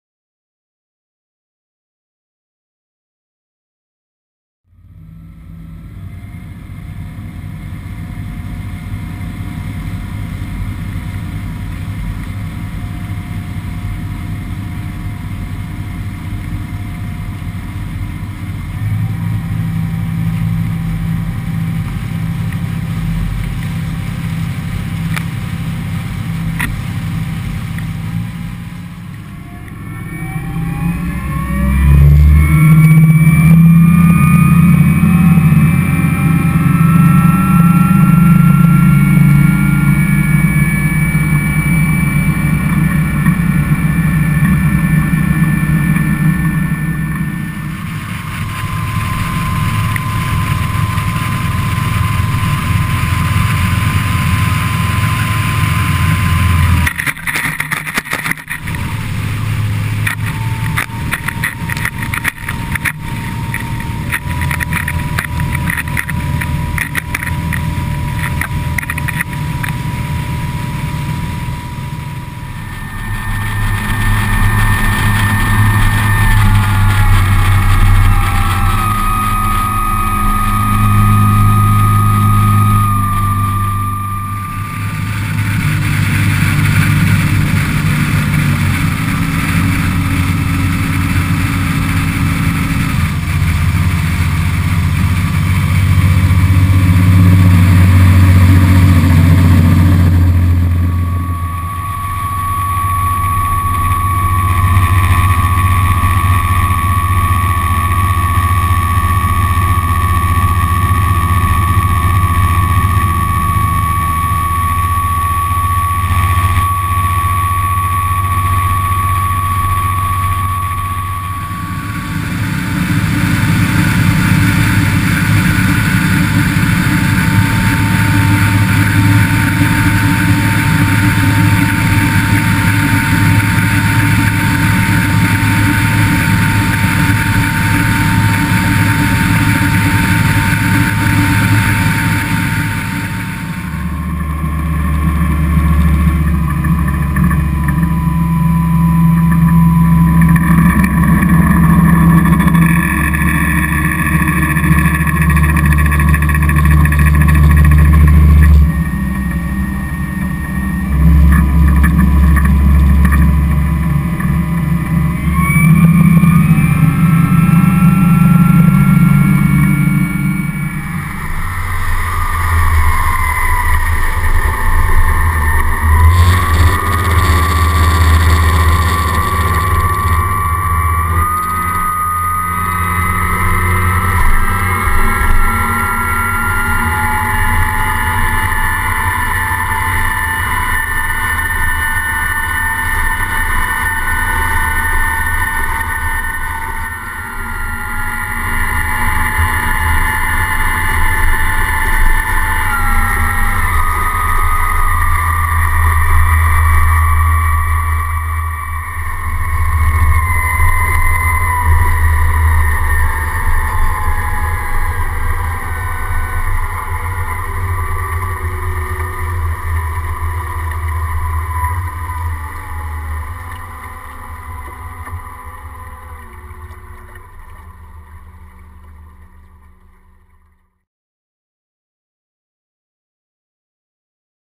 XO 270 RS - sunset run in Poole Harbour
Onboard footage from our sunset run in the XO 270 RS.  Fitted with the mighty Yanmar 370hp V8 diesel which provides the brilliant soundtrack!  Video taken in Poole Harbour, December 2013.